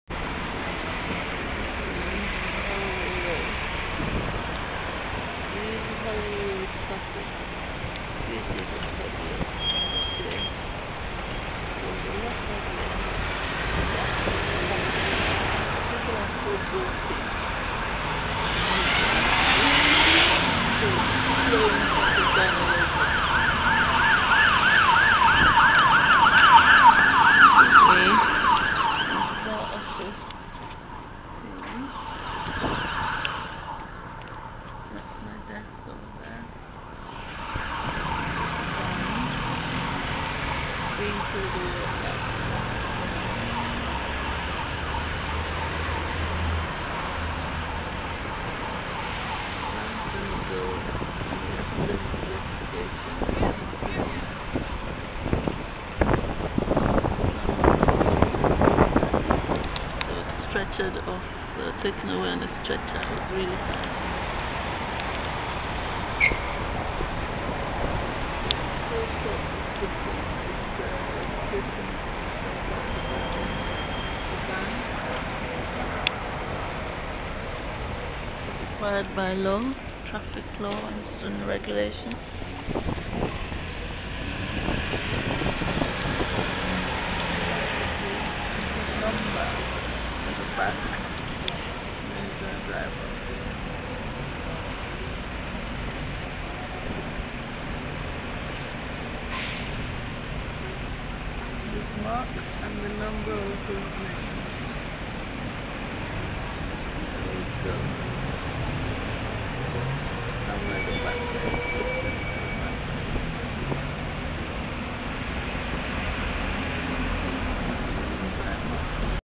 Holloway Road Bus Crash - The Van Is Towed Away
The van involved in the accident with Bus 43 on Holloway Road today is being prepared for towing by a tow truck driver.